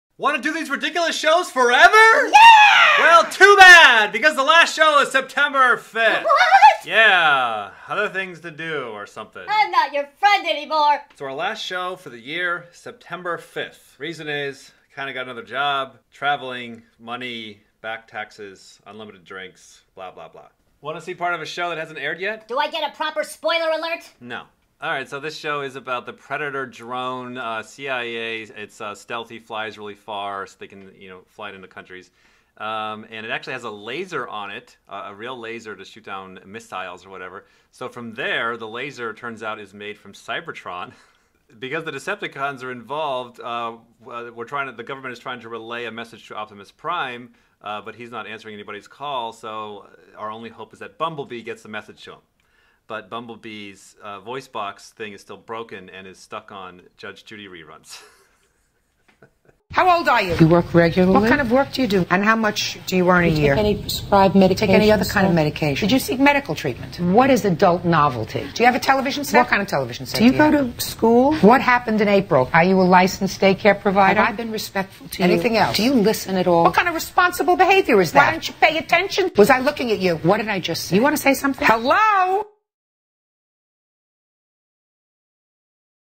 LAST SHOW (Sep 5th)
I got to put these shows on hold for the rest of the year. Work related stuff. All good things, just going to be busy traveling and working on boring business stuff off line. These videos are so fun to do, I hope to do more next year! Love Dave.